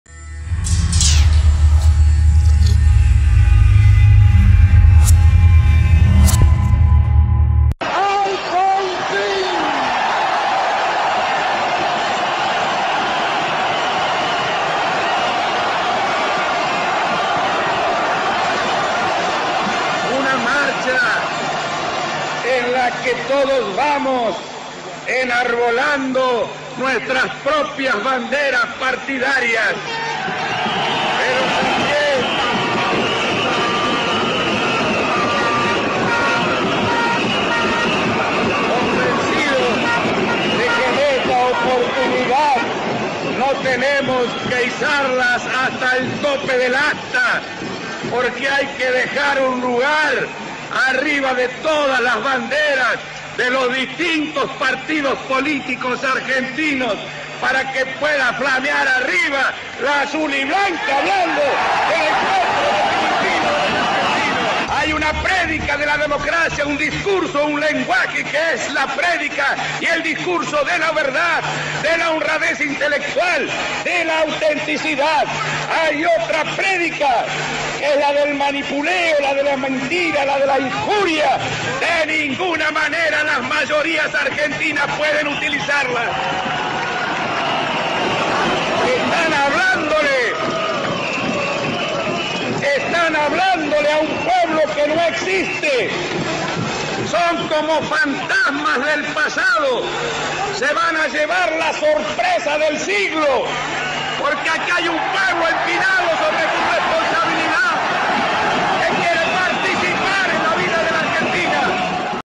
Raul Alfonsin 30 años de democracia
El 30 de octubre de 1983 fue electo presidente Raúl Alfonsín. Ese día quedó para la historia porque fue la primera elección democrática en siete años y el comienzo de una etapa que no se cerró.
Su discurso estuvo centrado en la defensa del Estado de Derecho y la importancia de respetar la Constitución Nacional; por eso, uno de sus latiguillos de campaña fue citar el preámbulo de la Carta Magna. Refundar la vida democrática argentina sobre un nuevo pacto social: esa era su idea-fuerza. "El pueblo unido, sin distinciones entre peronistas y antiperonistas, radicales o antirradicales, hará su tarea para defender los derechos de todos", gritó ante una ovación.
Enfrento mas de 10 paros generales (huelgas) de la CGT peronista que hizo que fracasara su Plan Austral y que se adelanten las elecciones, que Menem gano para que el país pierda todo.